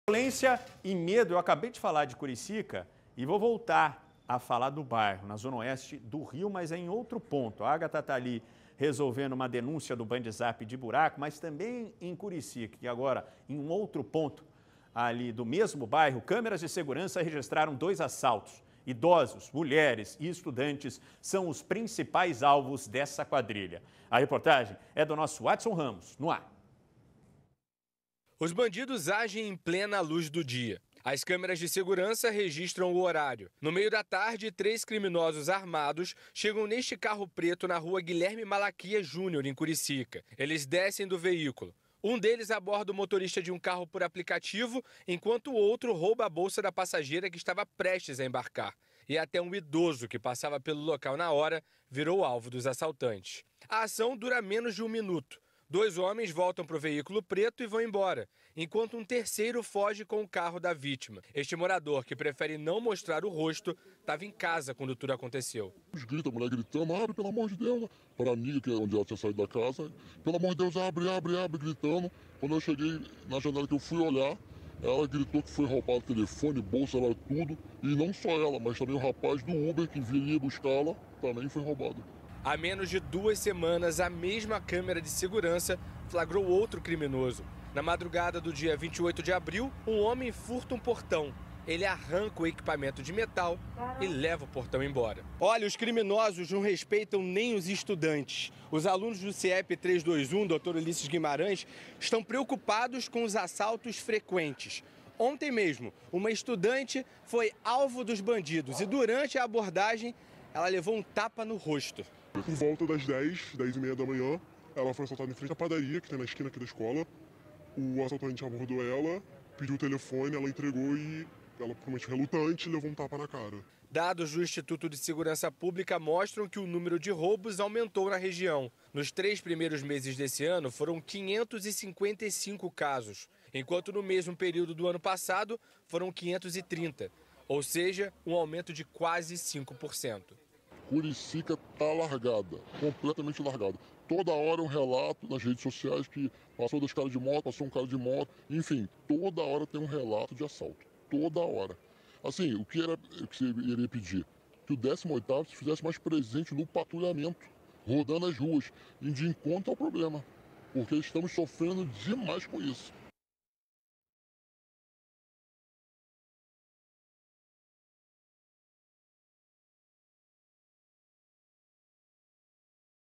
Violência em Curicica: assaltos recorrentes na região
Câmeras de segurança registram dois assaltos na mesma região. Quadrilha tem como alvo principal idosos, estudantes e mulheres.